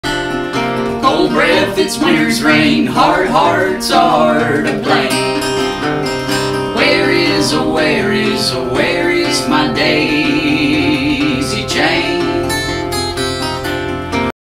14.5 seconds of interconnectedness between plants, animals, humans, and history, the daisy chain...
Acoustic folk harmonies fingerstyle and fingerpicked guitar. Is a term describing the seemingly unrelated yet interconnected web of life and past events.